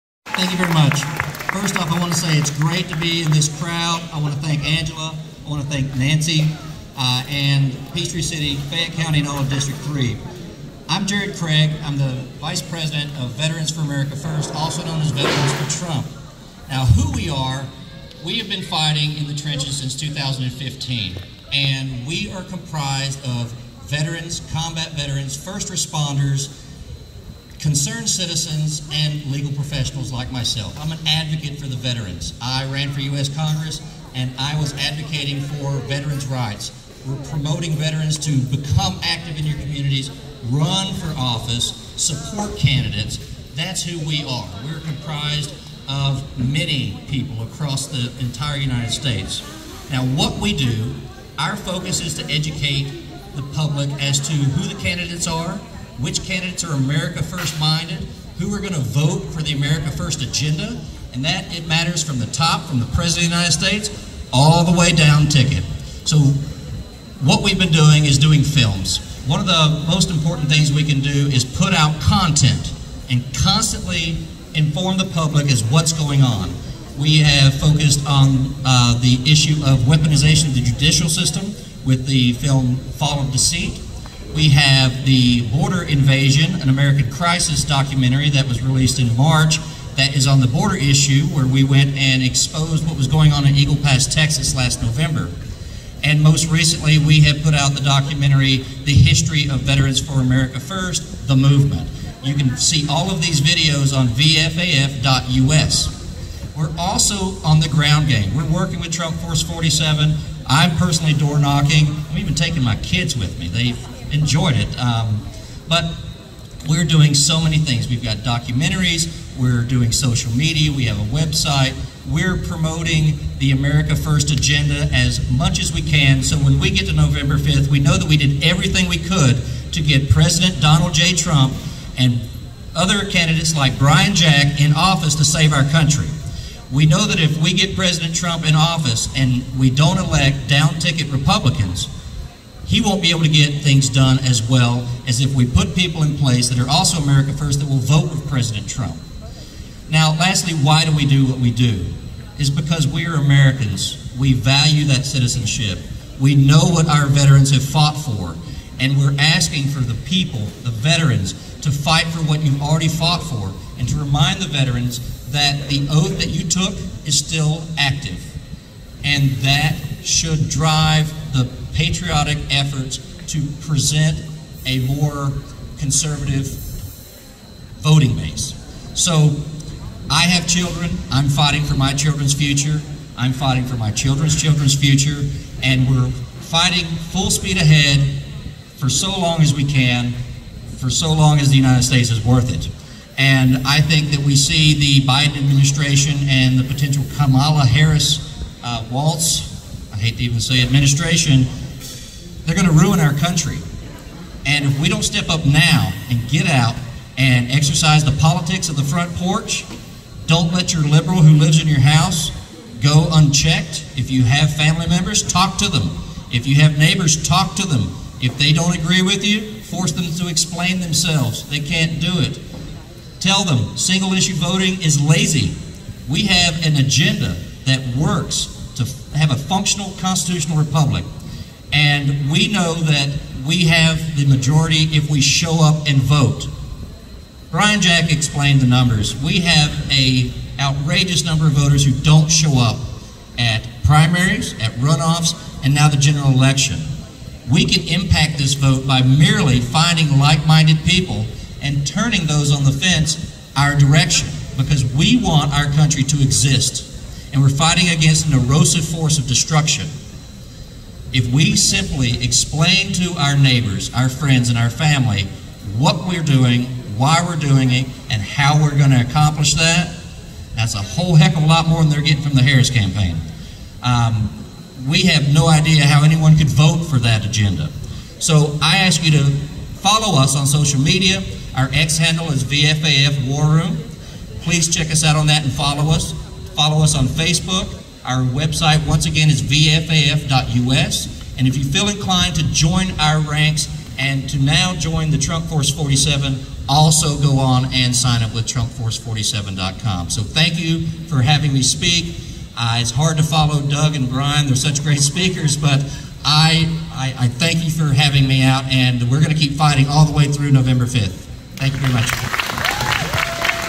Jared Craig, VP Veterans For Trump/ VFAF Speaks at Brian Jack Rally 9-21-24 in Peachtree City, GA
Jared Craig, VP of Veterans for Trump/ VFAF was a keynote speaker at the Vote America Rally hosted by the Fayette County Republican Party on September 21, 2024. Craig spoke to the crowd of over 200 Patriots and shared the history, current efforts, and goals of VFAF. Craig also highlighted the 3 educational documentaries: Fall of Deciet, Border Invasion: An American Crisis, and newest film, VFAF Veterans For Trump: The Movement available